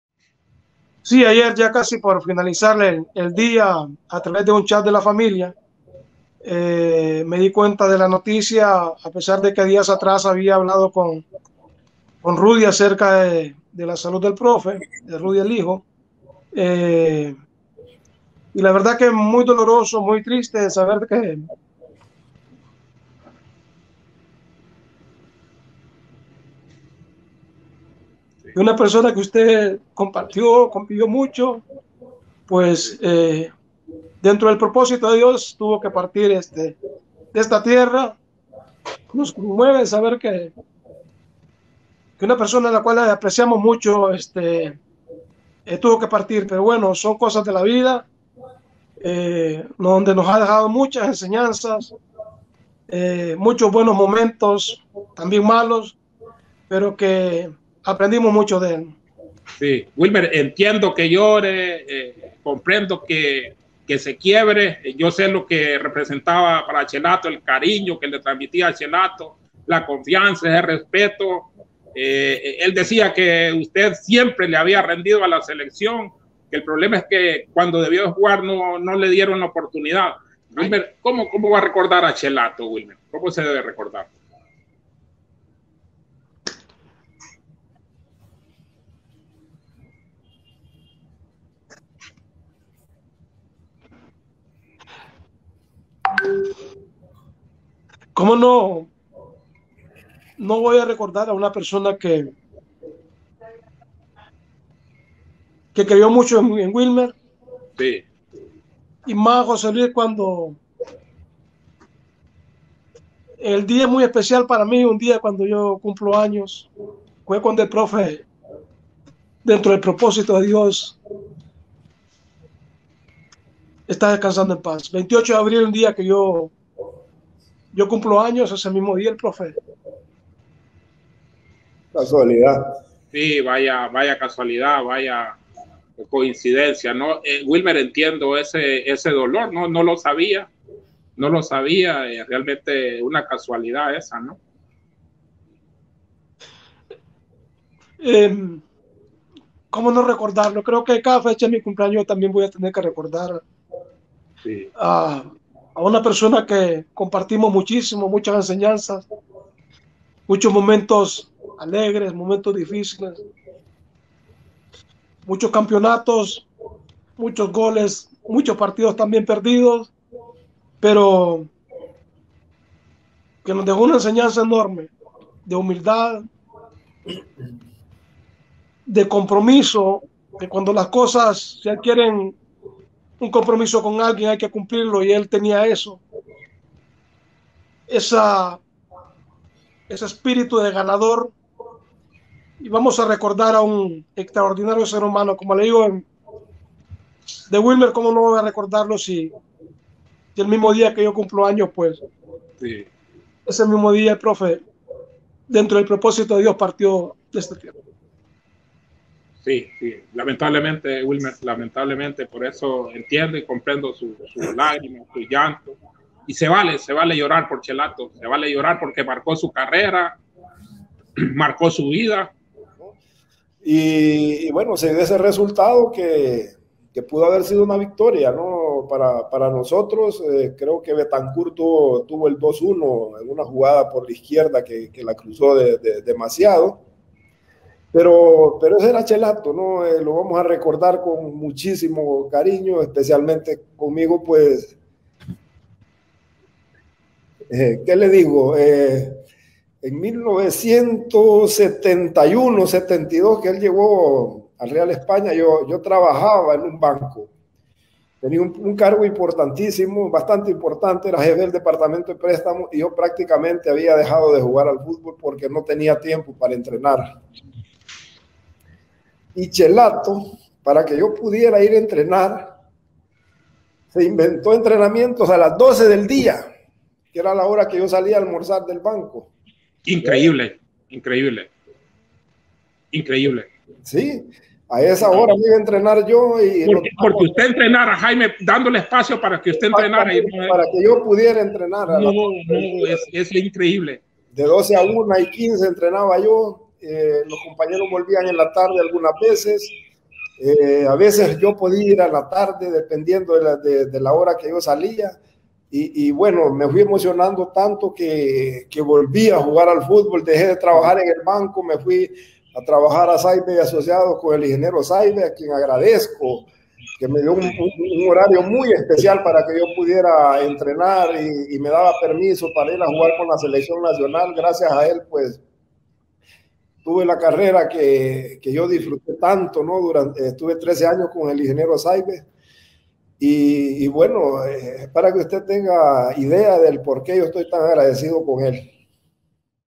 Wilmer Velásquez y Jaime Villegas rompen a llorar al recordar a Chelato Uclés
Cientos de futbolistas hondureños le deben gran parte del éxito de su carrera al profesor José de la Paz Herrera, conocido como Chelato Uclés, quien falleció este miércoles 28 de abril en Tegucigalpa.